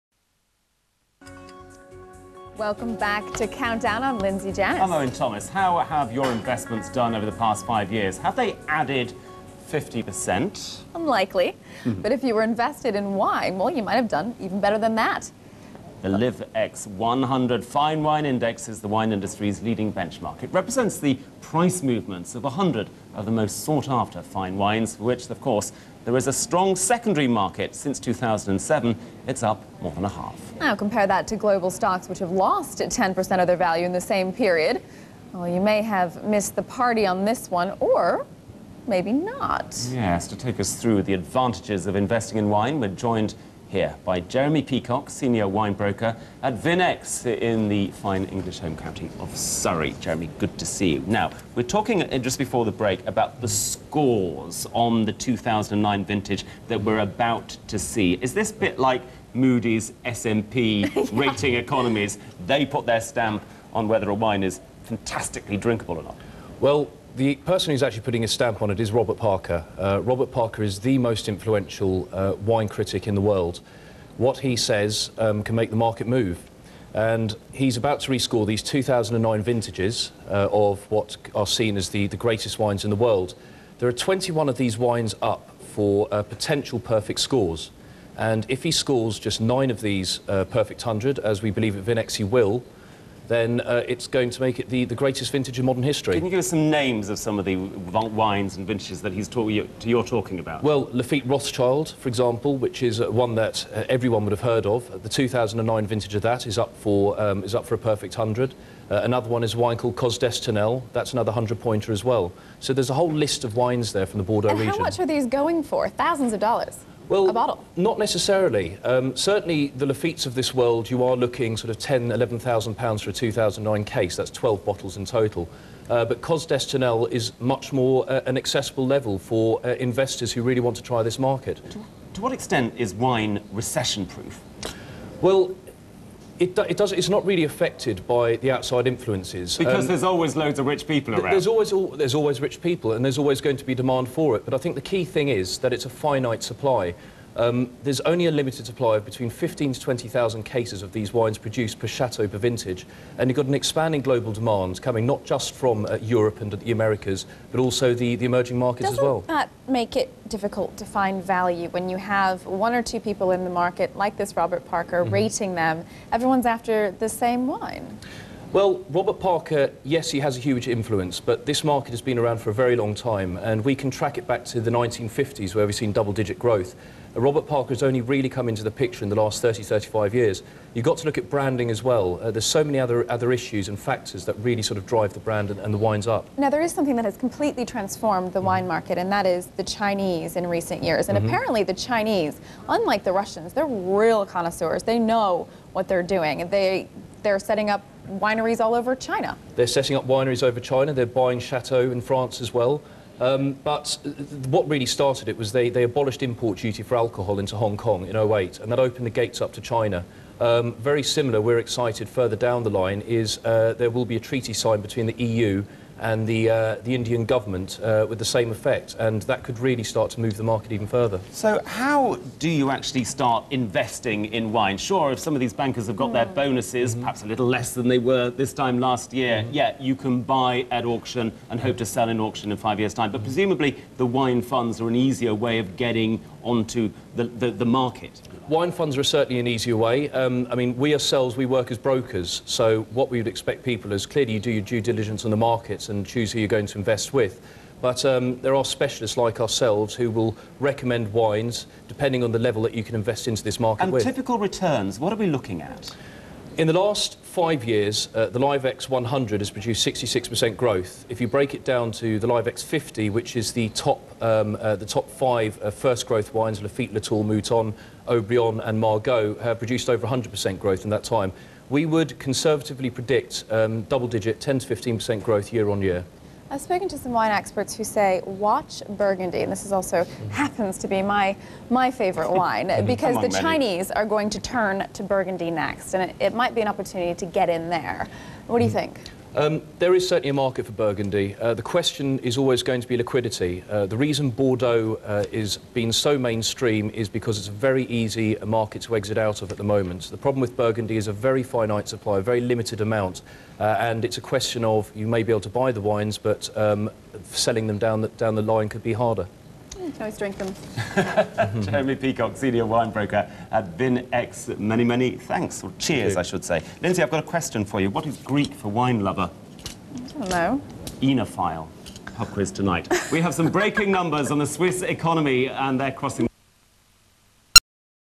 Vin-X Fine Wine Investment Brokers - Bloomberg TV interview with Senior Broker Jeremy Peacock
2009 Cos D'Estournel Good Buy for Wine Investors
An interview with Jeremy Peacock - A Senior Broker at Vin-X

The video above contains a Vin-X Bloomberg TV interview with Senior Broker Jeremy Peacock on the 'best ever' 2009 vintage (6.45am GMT 1st March 2012)